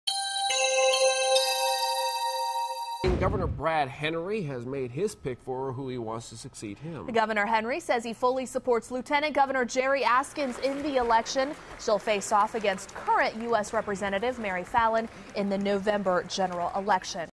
Gov. Henry Endorses Jari Askins
Gov. Henry has thrown his support behind Jari Askins for the Oklahoma governor seat.